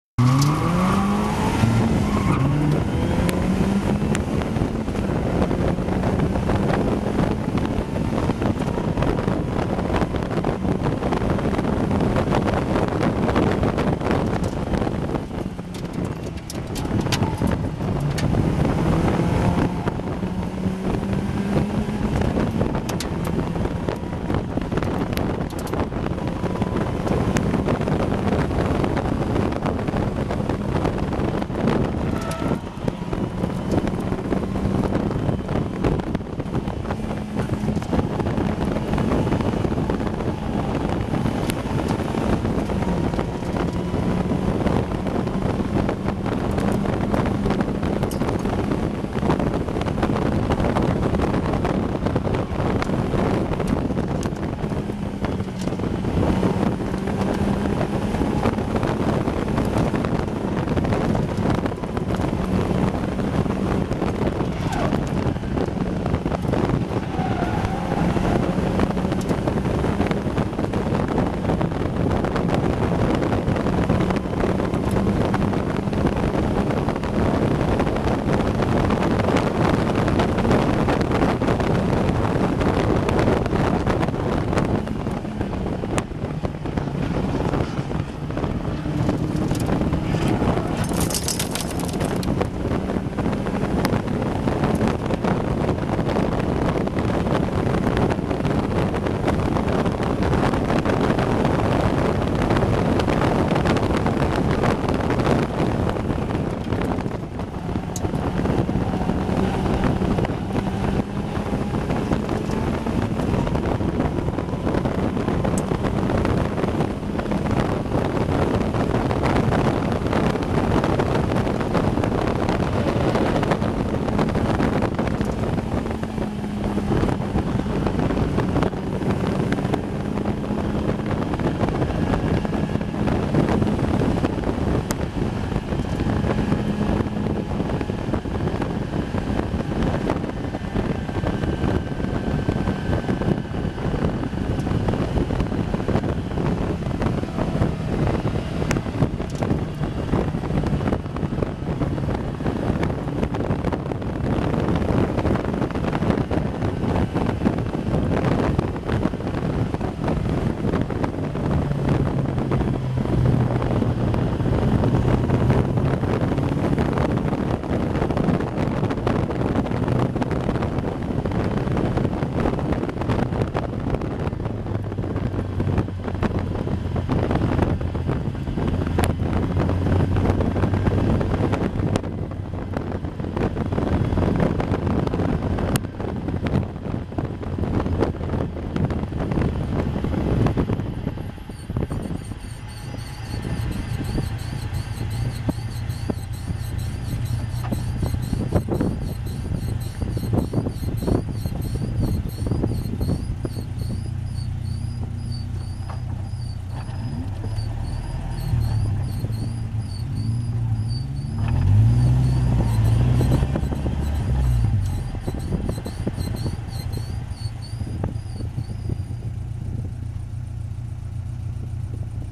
Caterham Roadsports at Donington - the race that wasn't
Here is the two-lap effort that preceded the second race of the weekend.  Sadly, Gavin span just ahead of me at the Esses (I'm sympathetic to that...!) leaving him in the middle of the track.  Most of us were able to get round him, but one wasn't and the result was a blocked track and a red flag stoppage.

A shame, as I had been doing well up to then, but more of a problem for the two drivers whose cars were damaged.  My sympathy to both of you...